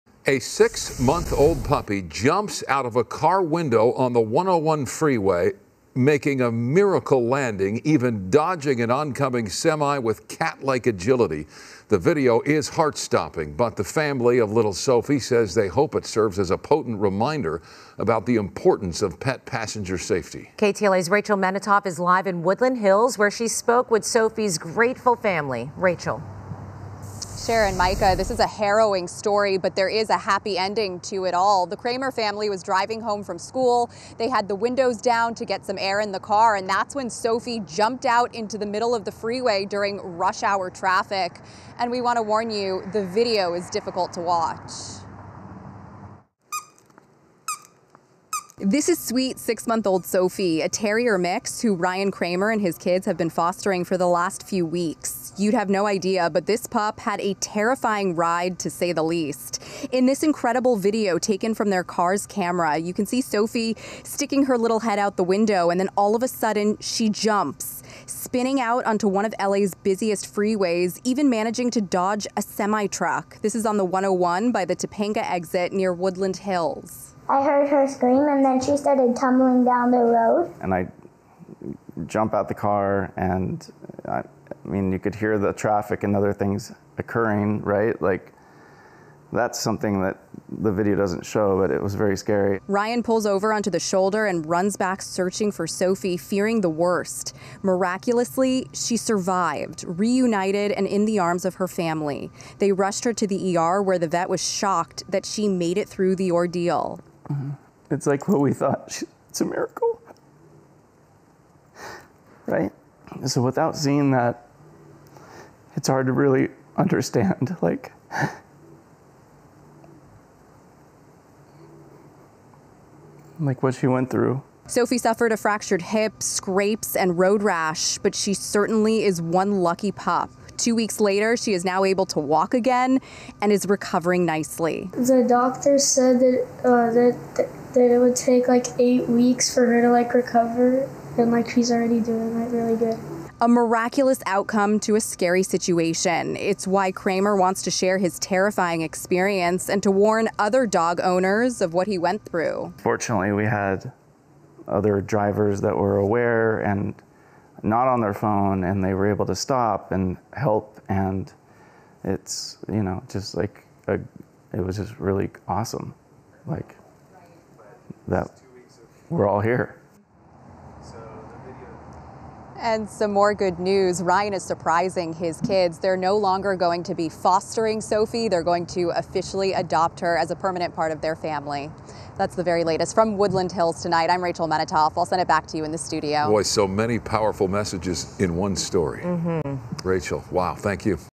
Dog miraculously survives after falling onto busy freeway in Los Angeles
Terrifying footage captured the moment a puppy leaps out of a moving vehicle on the 101 Freeway in Woodland Hills, California.

Video from a family’s Tesla cameras show six-month-old Sophie leaping from the window and tumbling into oncoming freeway traffic. The family is thanking their lucky stars that Sophie survived the terrifying ordeal.

KTLA's Rachel Menitoff reports on Feb. 10, 2023.